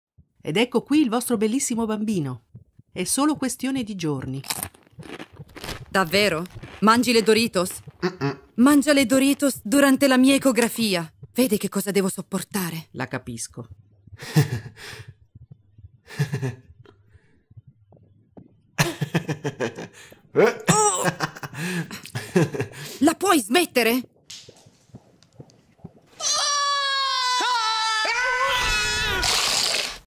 Pubblicità Doritos Super Bowl
Una ecografia mostra dei risultati interessanti in questa Pubblicità per le Doritos.

Doppiaggio completamente in italiano del video del Wall Street Journal "Super Bowl 50: Doritos Ad".